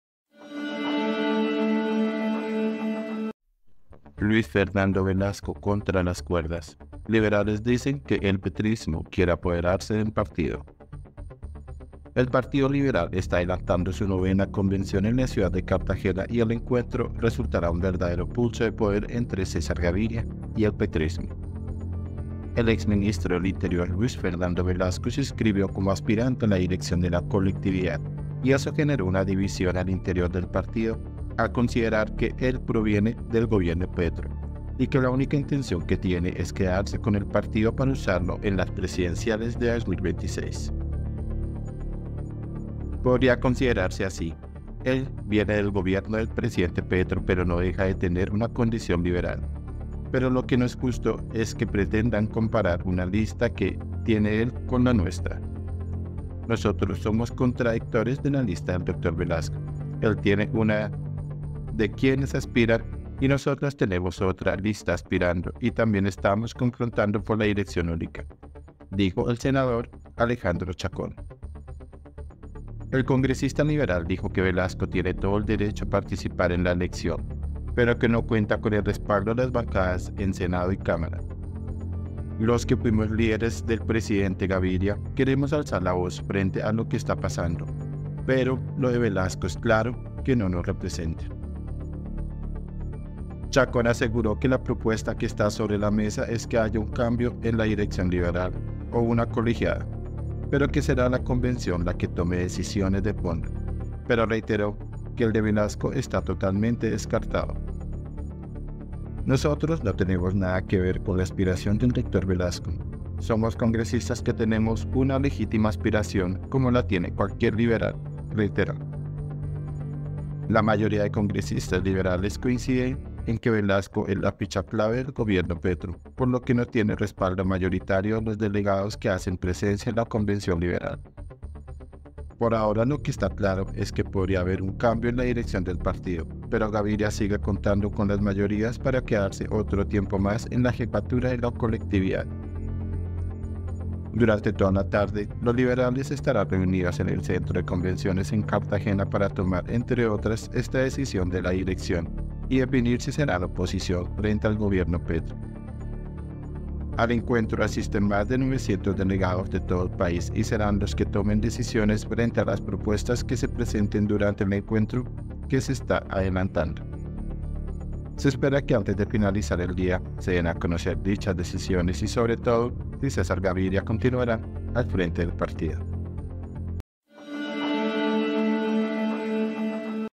LUIS FERNANDO VELASCO CONTRA LAS CUERDAS 🔴 “El petrismo quiere apoderarse del partido”
El exministro del Interior Luis Fernando Velasco se inscribió como aspirante a la dirección de la colectividad y eso generó una división al interior del partido al considerar que él proviene del Gobierno Petro, y que la única intención que tiene es quedarse con el partido para usarlo en las presidenciales de 2026.
SEMANA